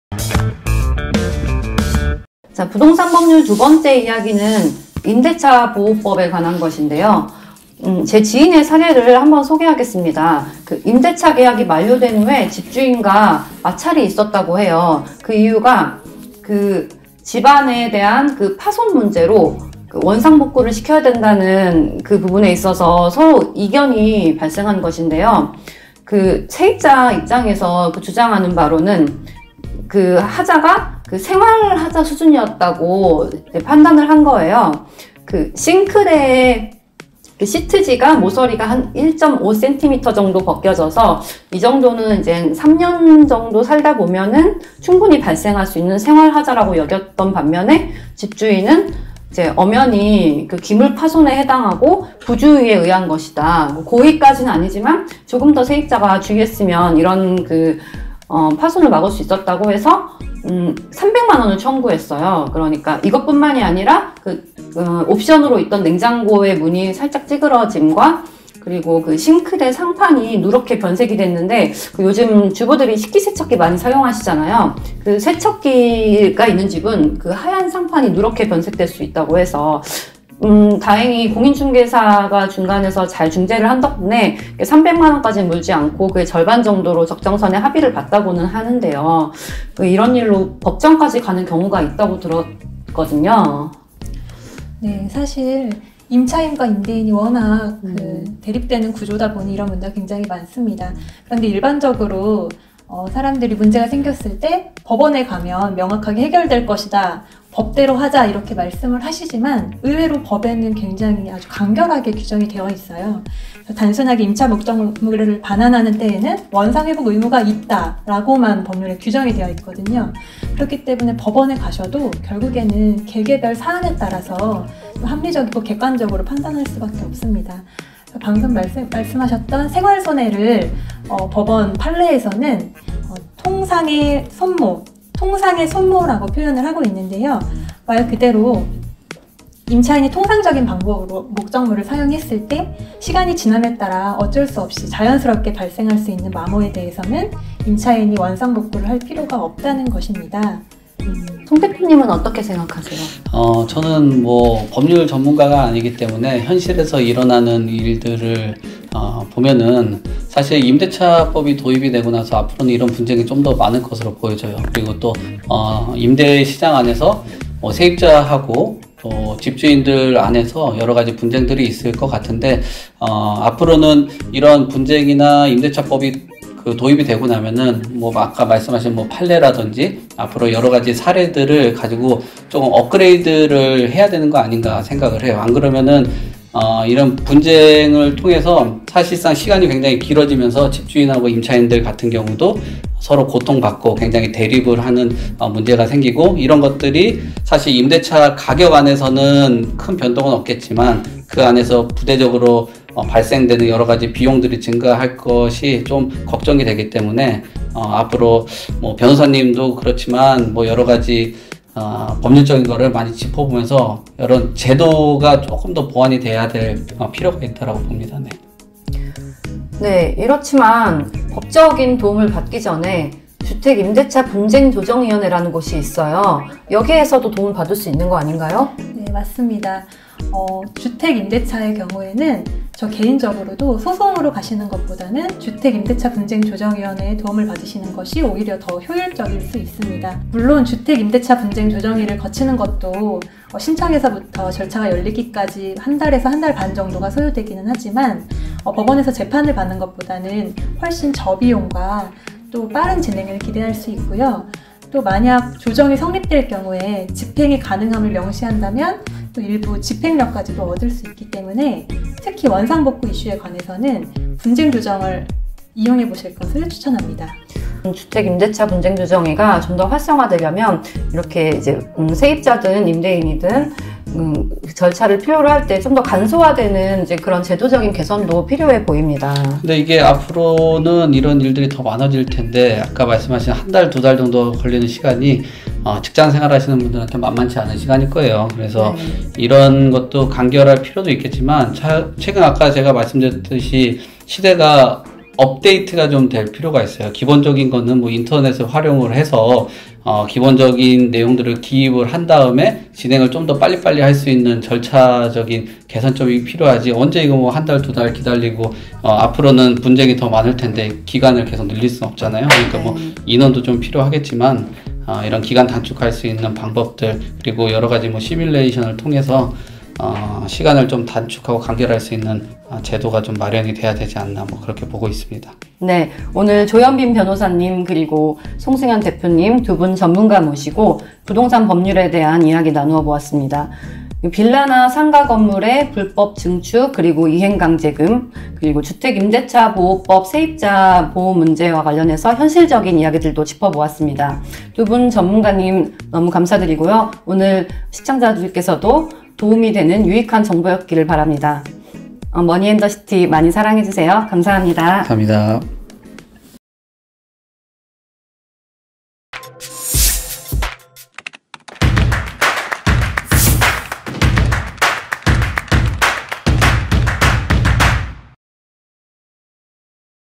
세입자의 원상복구 의무와 범위 [MONEY AND THE CITY]
적발시 건축법 제79조에 따른 시정명령(철거/원상복구) – 건축물대장 등재(일반건축물대장[갑] 노란색으로 위반건축물 – 법 제 80조 이행강제금(연2회까지) – 건축주 고발 벌금형

■ 주용남전문가의 부동산강의 ■
▶【오후 5시】
■ MONEY AND THE CITY ■
▶【화 목 오후 1시】
■ 어반스토리 ■
▶【특집편성】
■ 도시와경제 브이로그 ■
▶【대중없음】